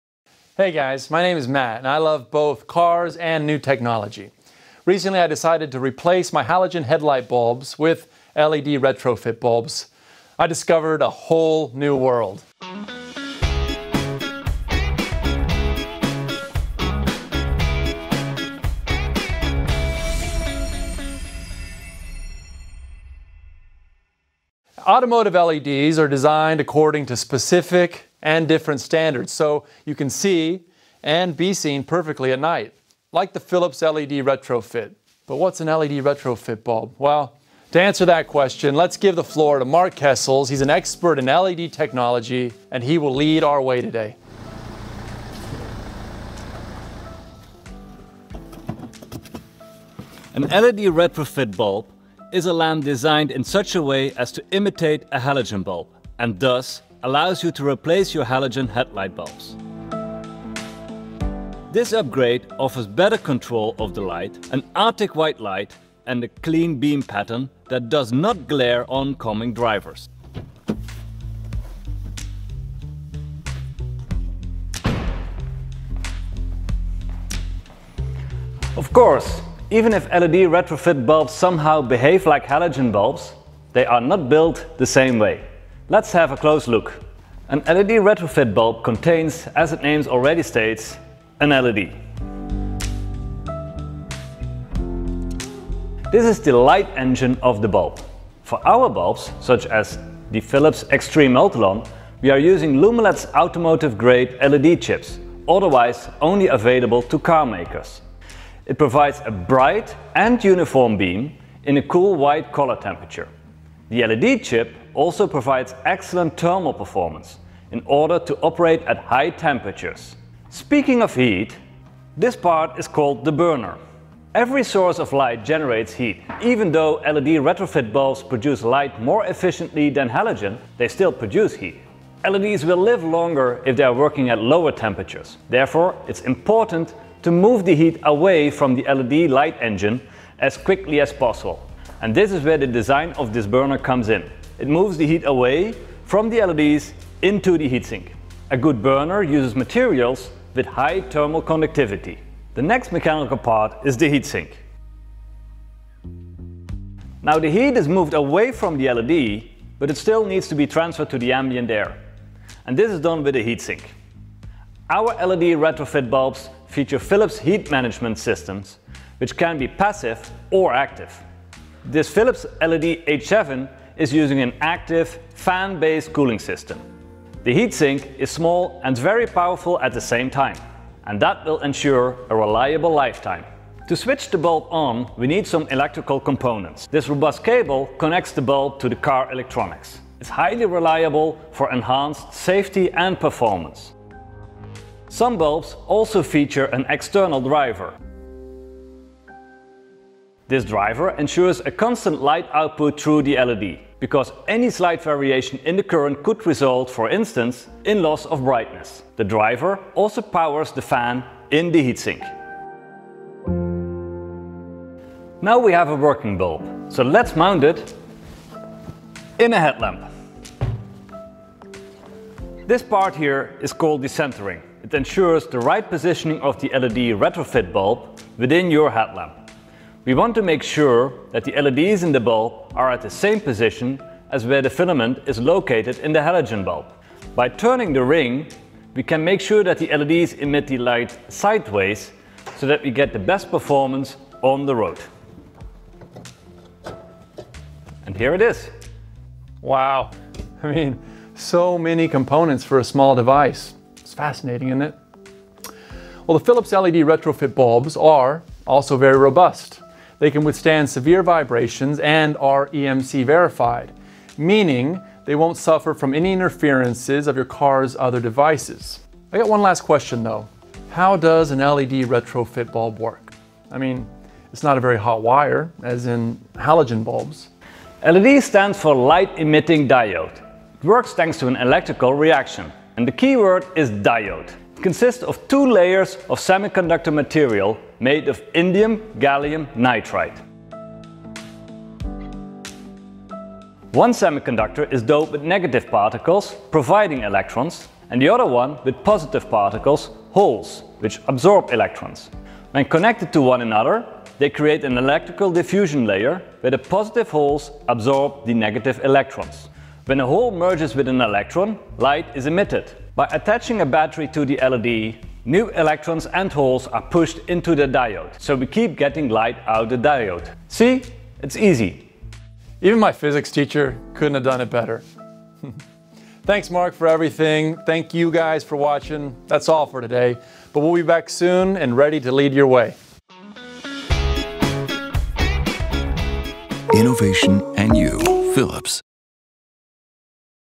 Co to jest żarówka LED typu retrofit?
Zastanawiasz się nad wymianą żarówek halogenowych na żarówki typu retrofit? Matt i nasi eksperci w dziedzinie oświetlenia samochodowego z firmy Philips pomogą Ci odkryć całkowicie nowy świat żarówek LED, ich technologię i sposób, w jaki mogą one zmienić Twoje wrażenia z jazdy.